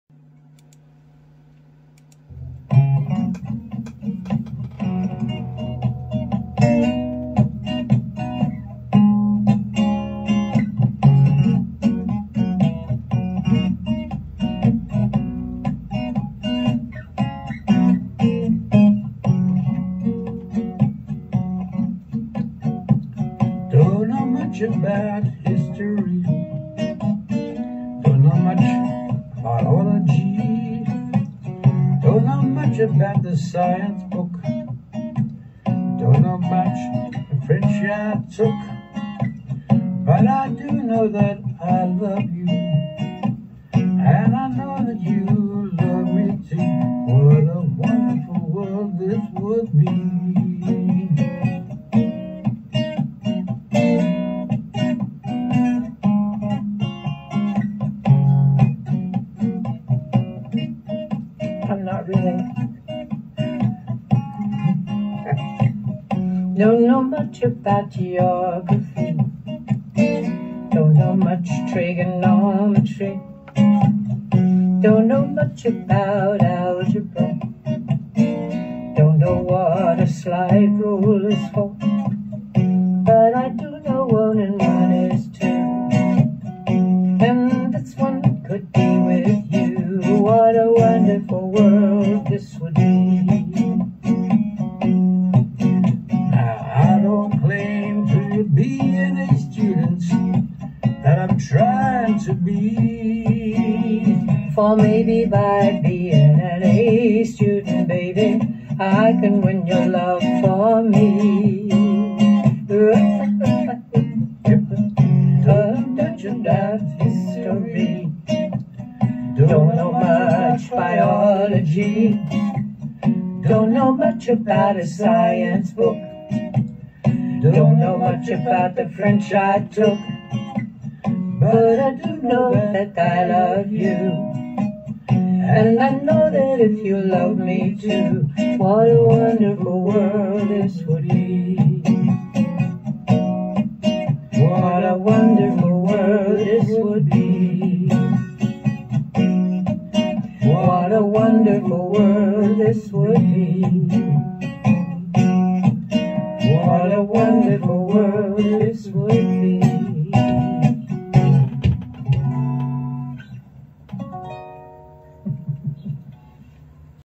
"Sam Cook"
what great songs Sam Cook sang.This one was such a big hit in the old days.just love it
Sang here by Ann  and Gib. I did not realize the sun was shining on the cell phone